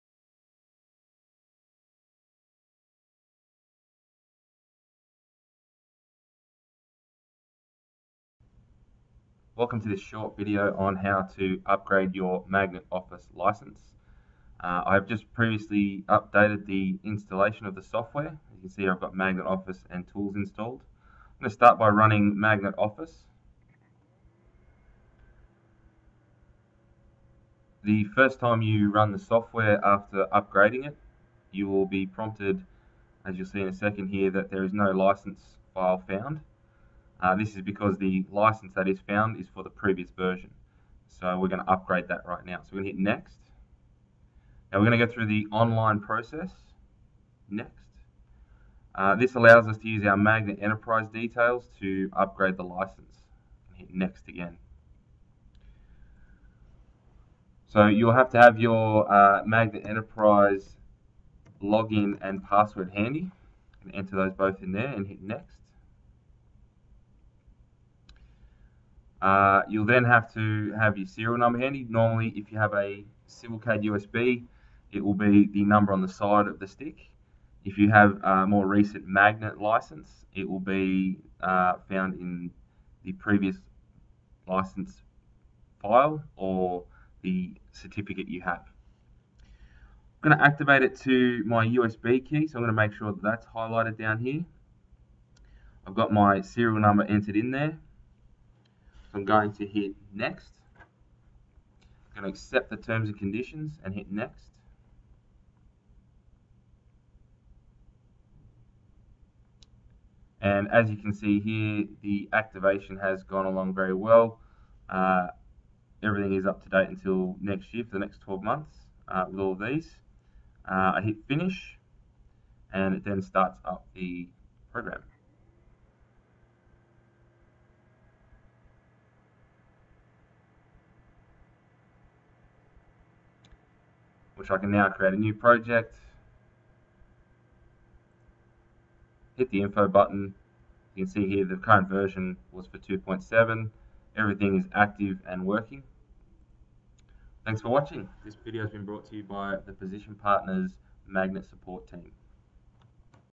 How to: Activate a new license in Magnet Office
Watch this short video to find out how to activate a new license in Magnet Office.